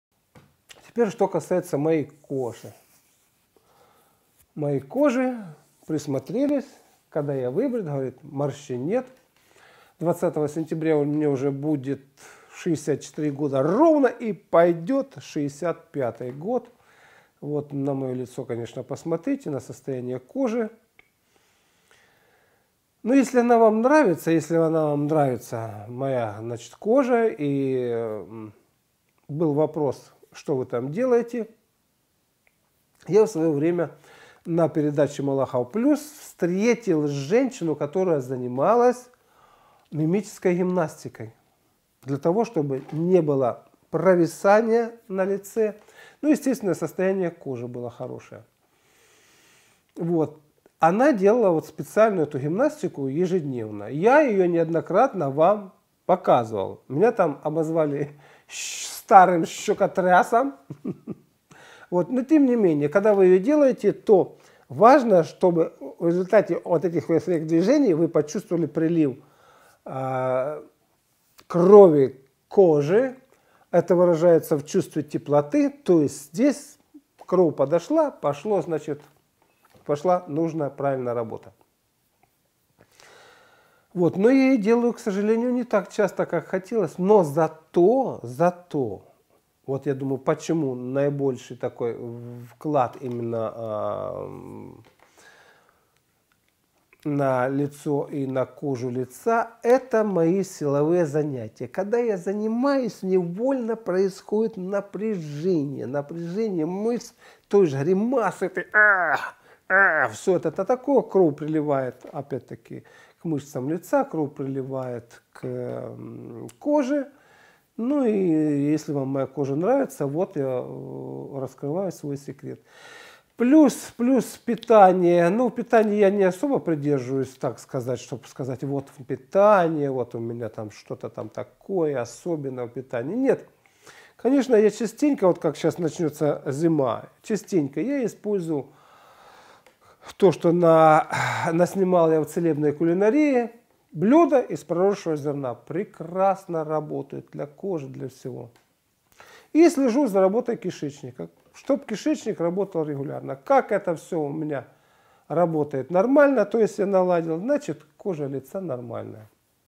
Кожа на лице - мой уход за ней.
Цикл передачо здоровом образе жизни. Автор идеи и создатель передач, Малахов Геннадий.